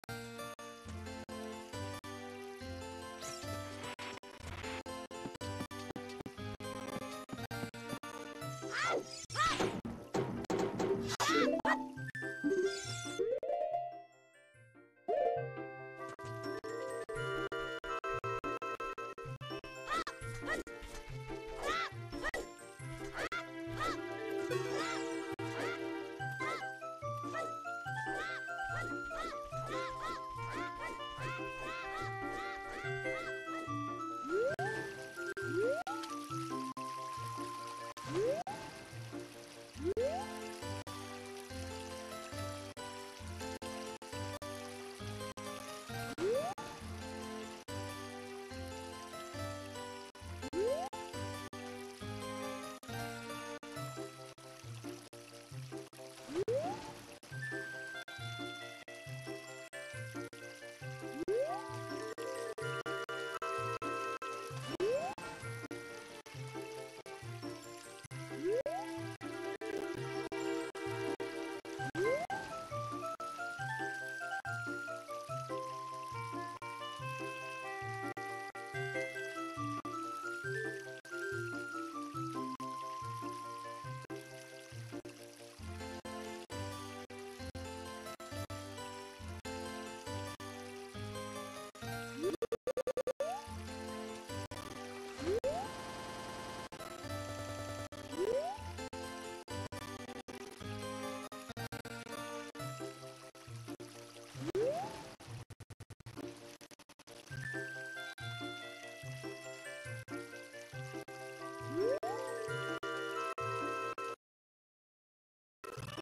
Walking while talking (WWT) + water = WTF
When you have WWT when you go in water you seem to just keep the speed that you went in at. lol.